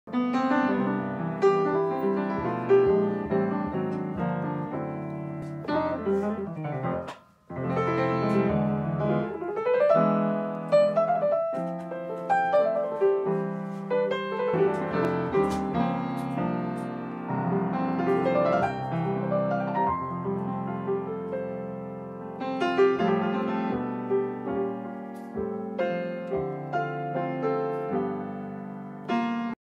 Yamaha Piano, The Best Sound!
Siempre disfruto sentarme a tocar un buen piano, pero el sonido de Yamaha Pianos siempre es Outstanding! Y se disfruta más...

Upright Piano, Grand Piano
Piano de pared, Piano de Cola